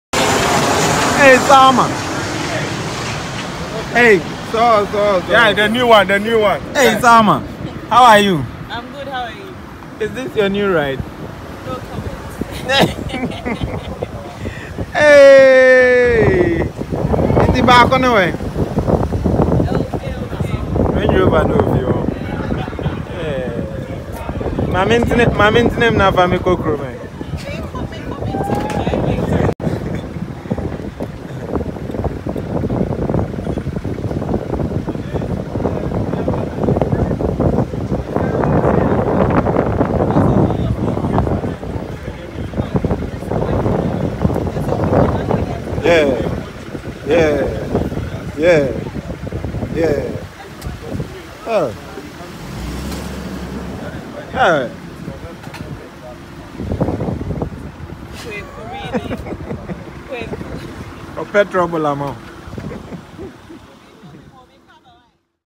WOW!!! Actress Salma Mumin Buys New Heavy Ride
Actress Salma Mumin grabs new ride and adds to her collection of cars. She recently bought white Range Rover about 7months ago. Check it out.